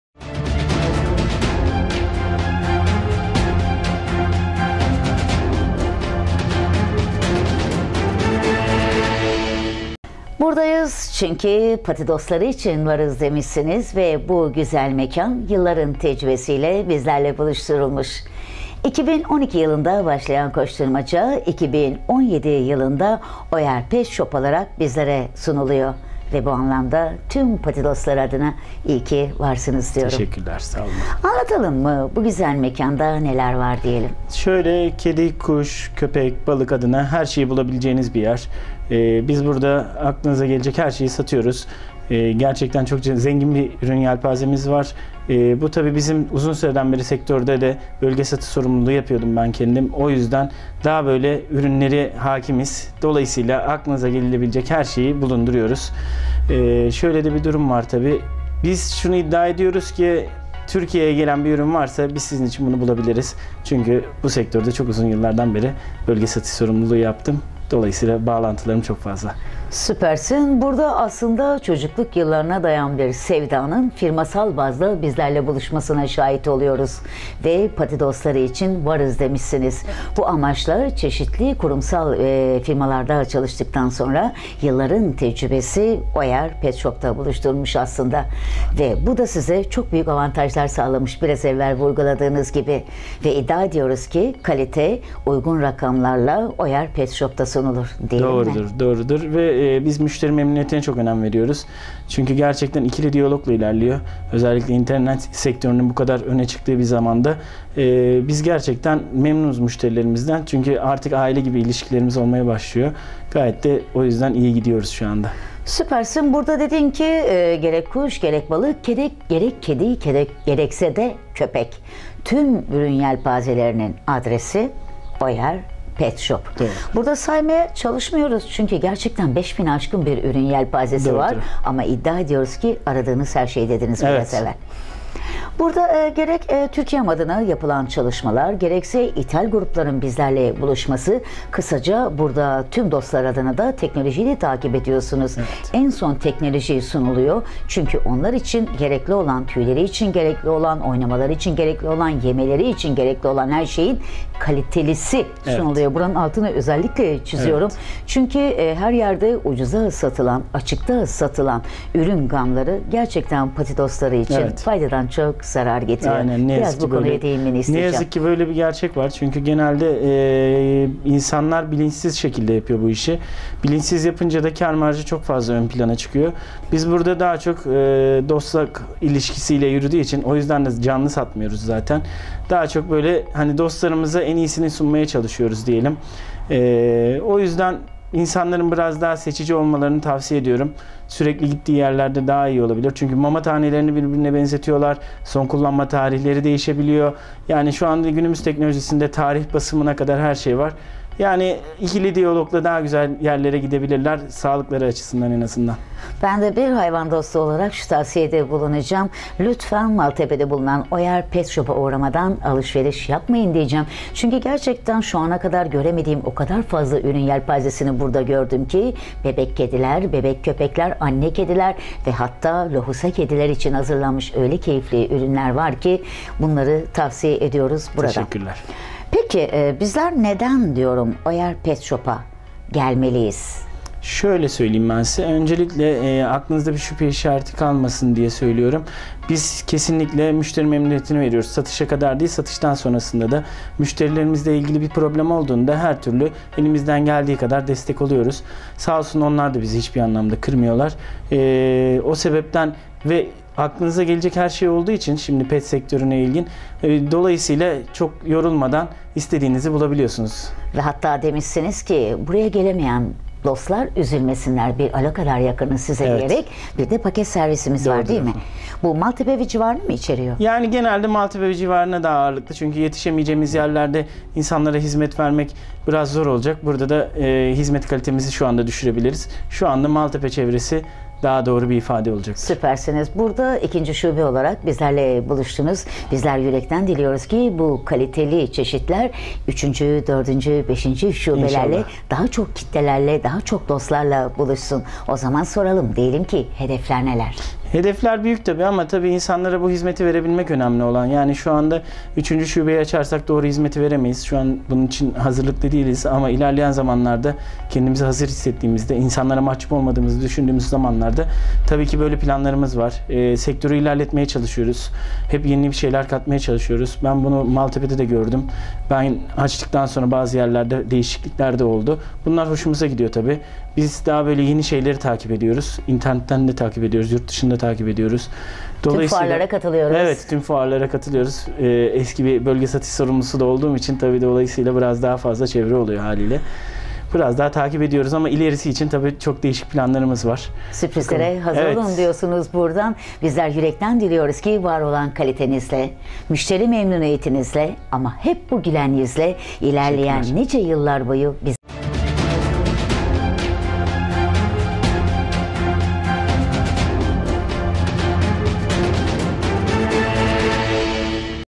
oyer petshop
MALTEPE İSTANBUL Faaliyet gösteren “ oyer petshop   “   firma sahibi ERGİN BANKOĞLU   sektördeki başarı ve hizmetlerini bugün kobi özel’e anlattı.